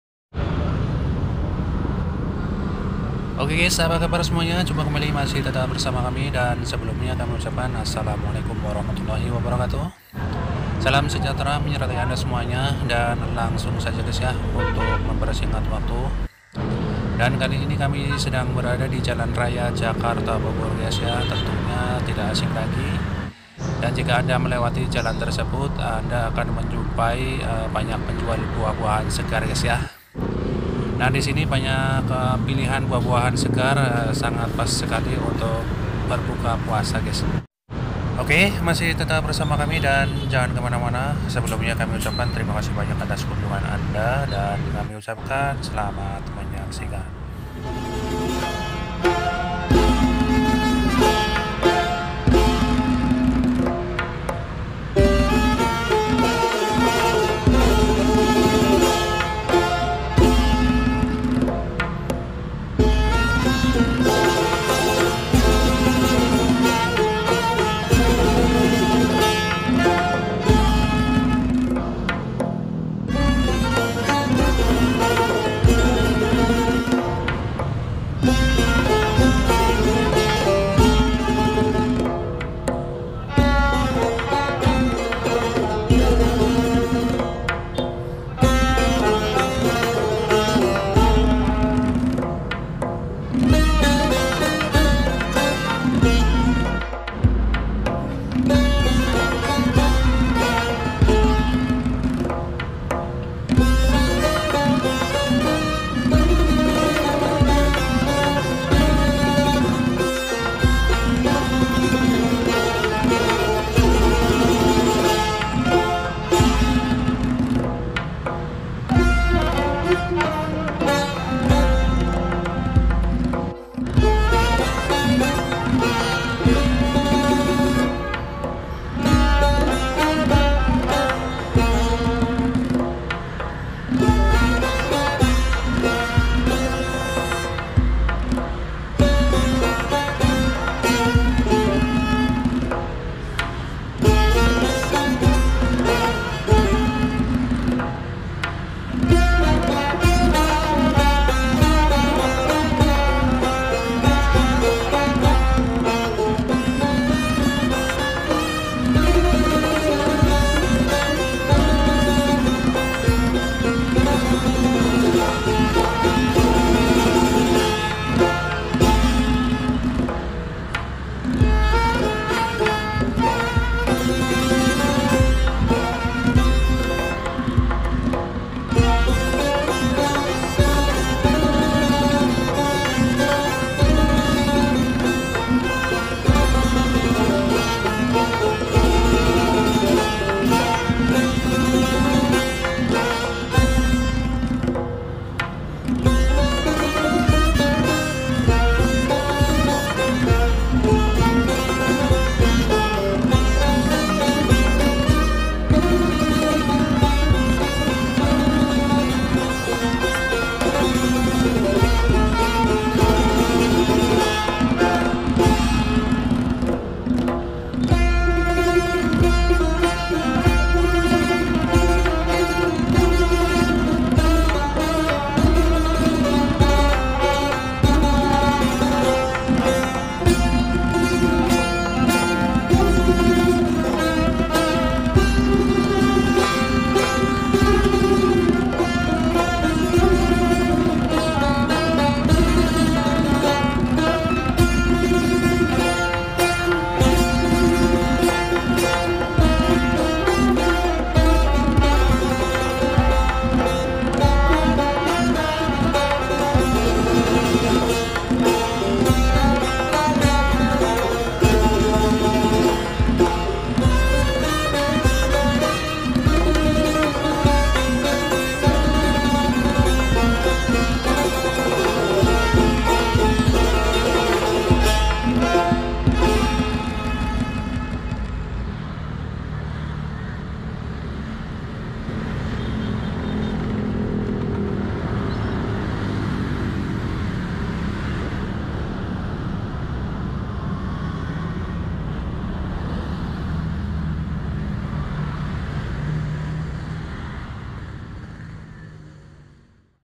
BUAH SEGAR
Penjual buah buahan  di sepanjang jalan anda pasti mengenal dan tidak asing lagi jika anda melewati jl raya jakarta bogor , banyak pilihan buah2an segar dan pas untuk teman berbuka puasa dgn harga yg terjangkau.